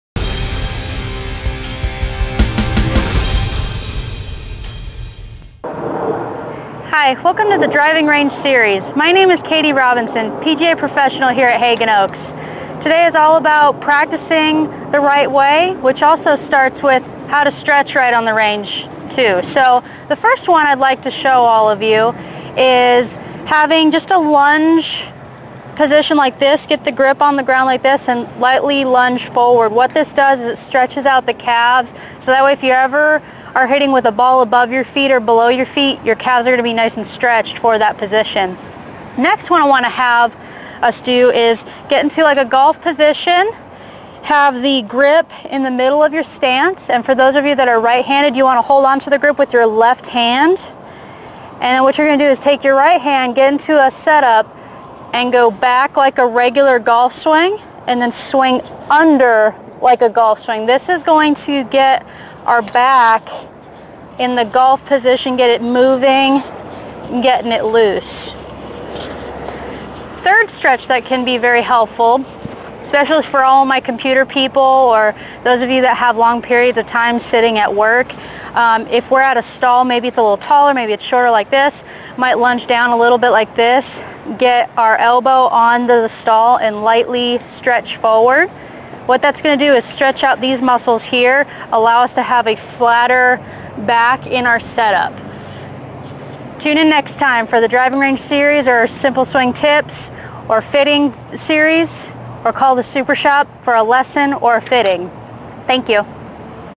How to Stretch Before your Round of Golf
PGA Professional, Katie Robinson demonstrates a few stretches that are important before playing golf.